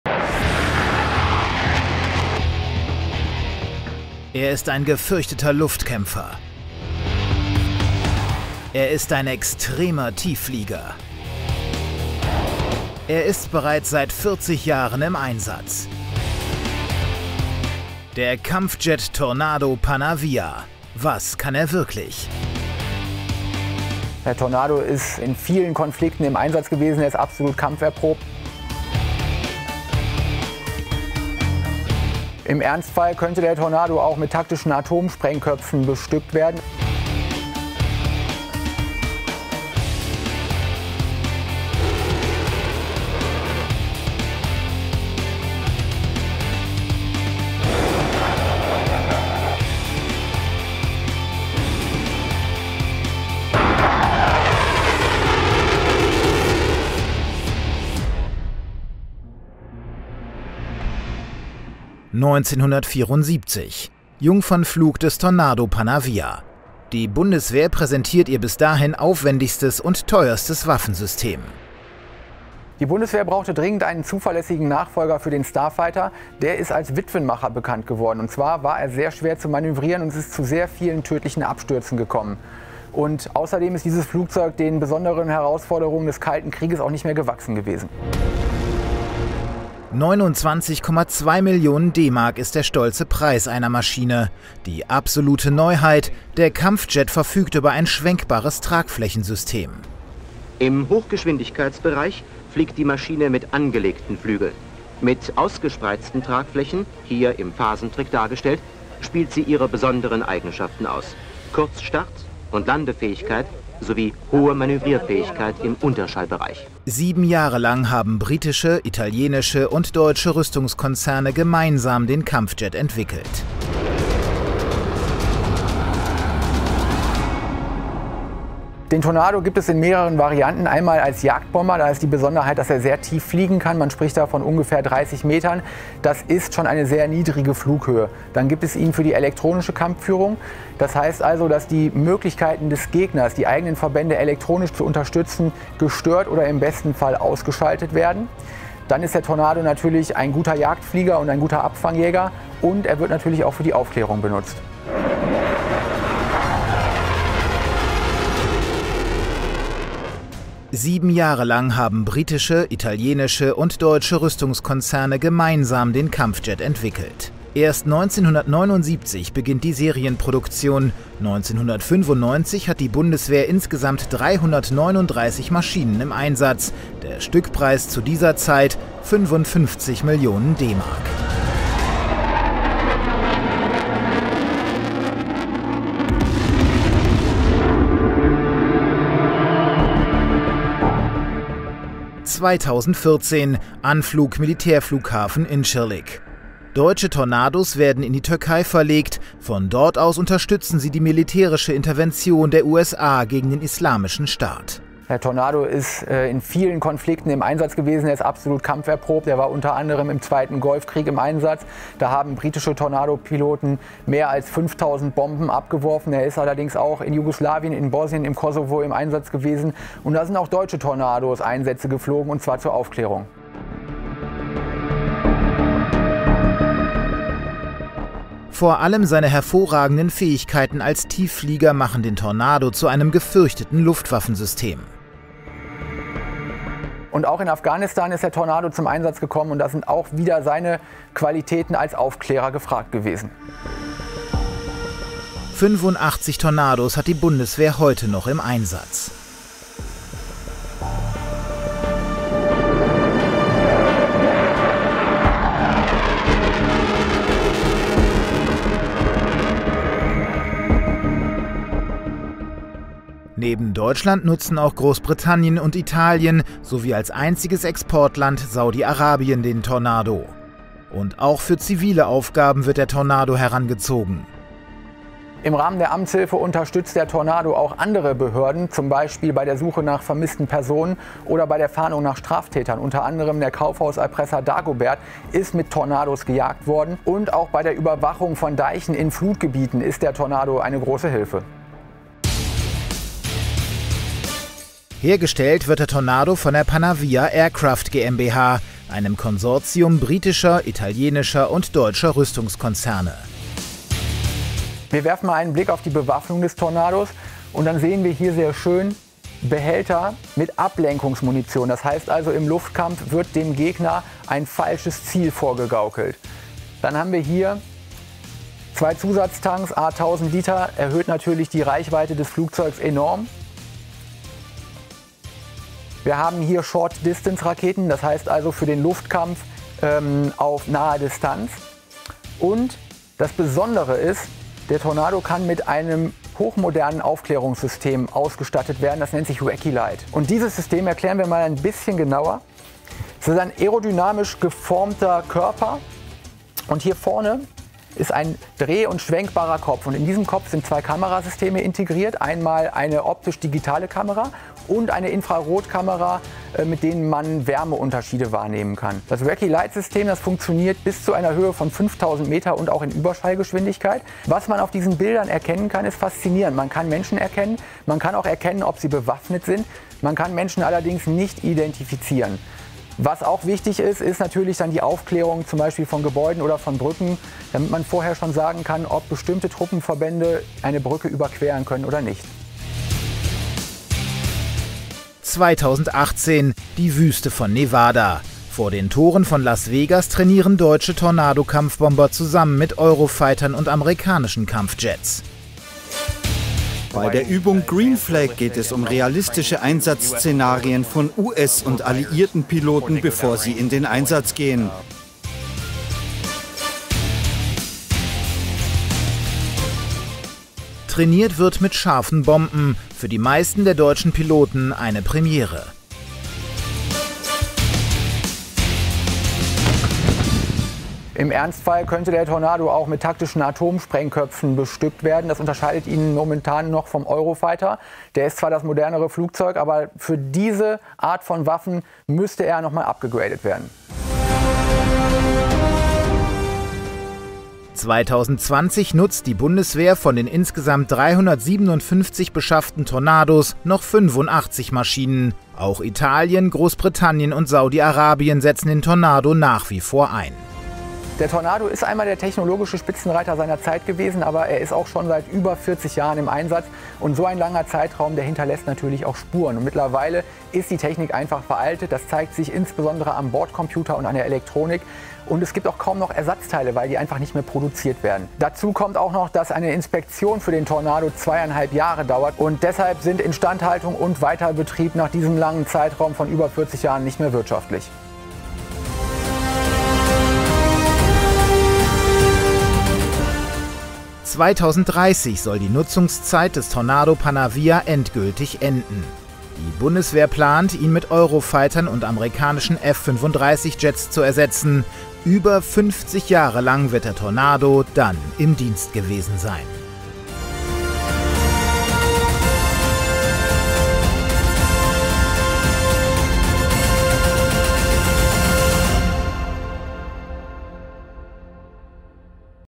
Panavia Tornado: Deutschlands einziger atomwaffenfähiger Kampfjet
Der Panavia Tornado ist Deutschlands einziger atomwaffenfähiger Kampfjet. Seit seinem Jungfernflug im Jahr 1974 ist er ein gefürchteter Luftkämpfer und Tiefflieger. Die Bundeswehr suchte einen zuverlässigen Nachfolger für den Starfighter und entwickelte den Tornado in Zusammenarbeit mit britischen, italienischen und deutschen Rüstungskonzernen. Der Tornado wird in verschiedenen Varianten eingesetzt, darunter als Jagdbomber, für elektronische Kampfführung und Aufklärung. Er war in vielen Konflikten wie dem zweiten Golfkrieg, Jugoslawien, Bosnien, dem Kosovo und Afghanistan im Einsatz. Deutschland, Großbritannien, Italien und Saudi-Arabien nutzen den Tornado, auch für zivile Aufgaben wie Amtshilfe und Überwachung. Der Tornado wird von der Panavia Aircraft GmbH hergestellt und verfügt über verschiedene Bewaffnungen sowie ein hochmodernes Aufklärungssystem. Obwohl der Tornado über 40 Jahre im Einsatz war, ist seine Technologie veraltet und die Ersatzteilversorgung schwierig. Die Bundeswehr plant, ihn bis 2030 durch Eurofighter und amerikanische F35-Jets zu ersetzen.